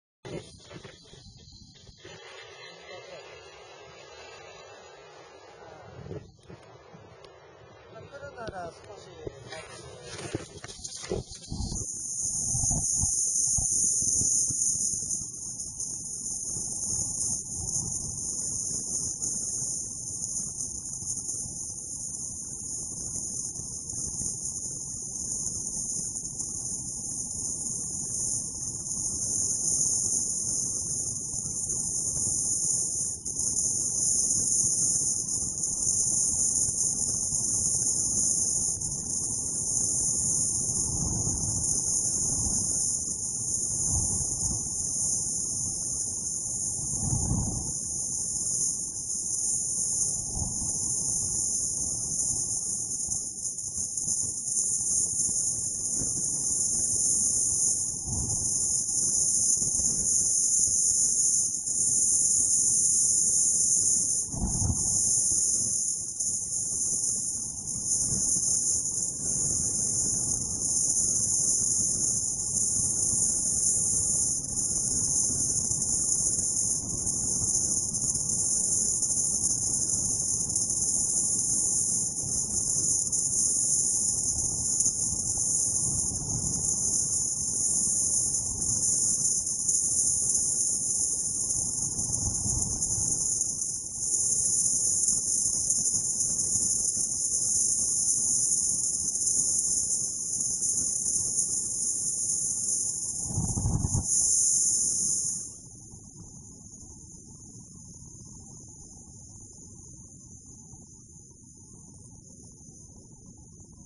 090405国際1143～サラブレッド
気温の高さから止むを得ないが、かなりのザクザク雪。最終日で滑走者が少なかったので荒れは余り無かった。それなりに良い感じでラストランを楽しめた。